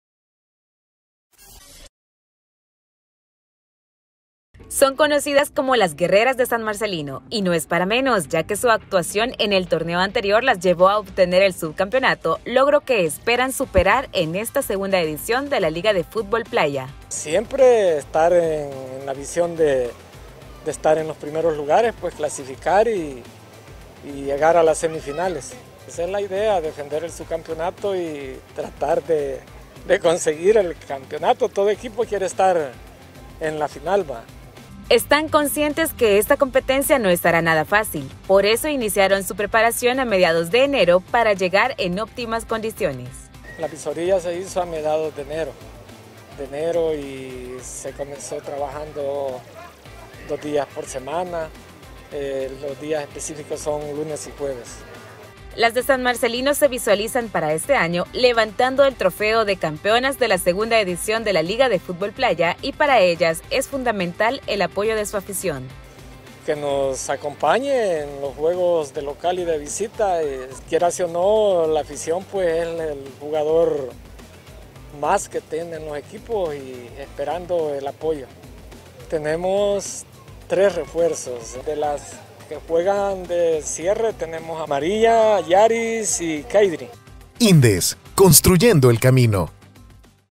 San Marcelino quiere levantar la copa
Para San Marcelino este torneo plantea un reto interesante, pues tiene como objetivo superar la actuación que llevó al equipo a quedarse con el subcampeonato de la I Copa CEL, y no es para menos, ya que en el torneo pasado hicieron honor a su apodo: Las Guerreras de San Marcelino.